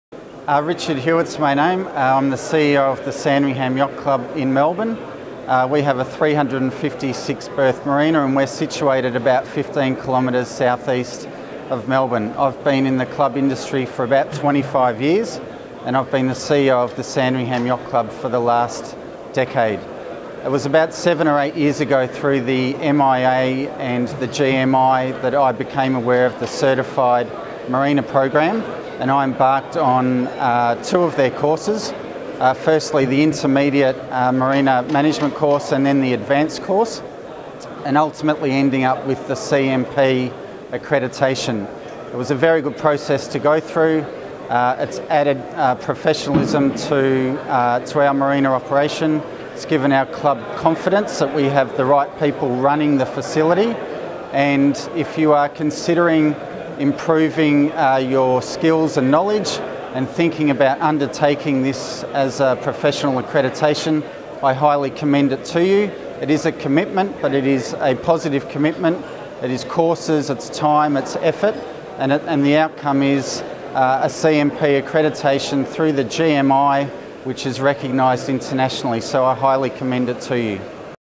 Richard Hewett - CMP
Richard Hewett discusses CMP certification and its benefits